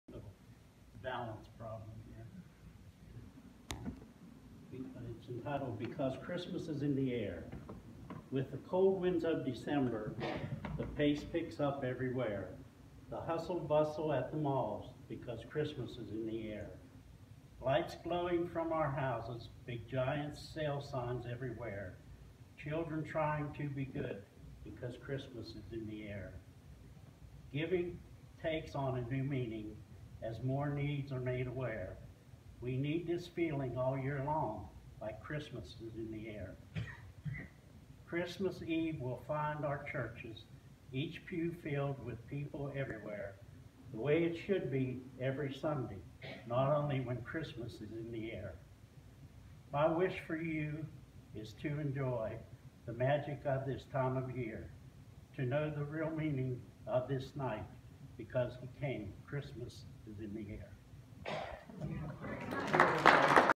Poem “Christmas is in the Air” 2017-12 Hymn Sing
12/10/2017 Arnold Friend with a poetic treat at Greene County United Methodist Hymn Sing at the Bobtown United Methodist Church.